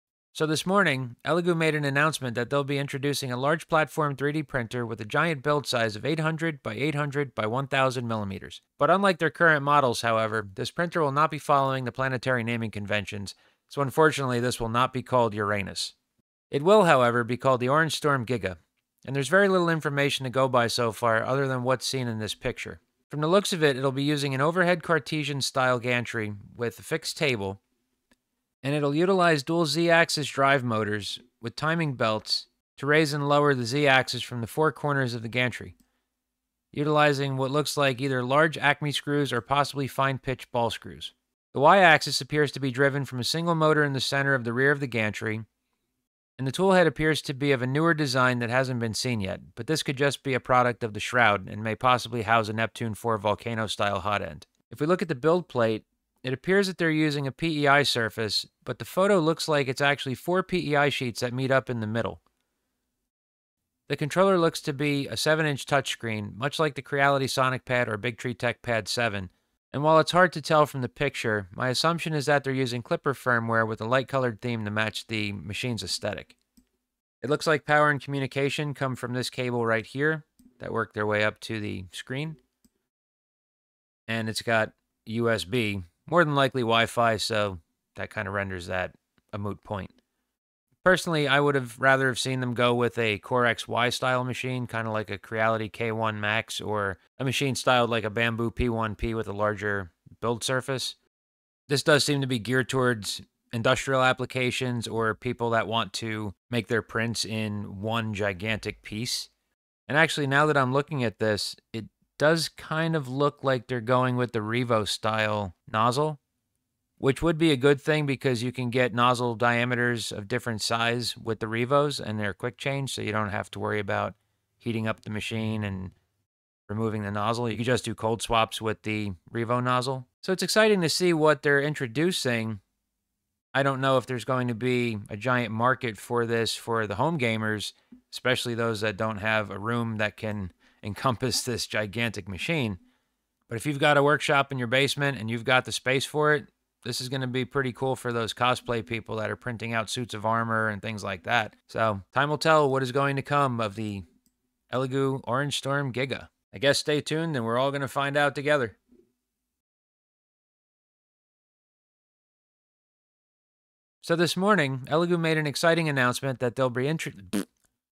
New product announcement from Elegoo - OrangeStorm Giga
Elegoo's first foray into the industrial 3D printing space looks pretty good, even though the name isn't what I had hoped it would be...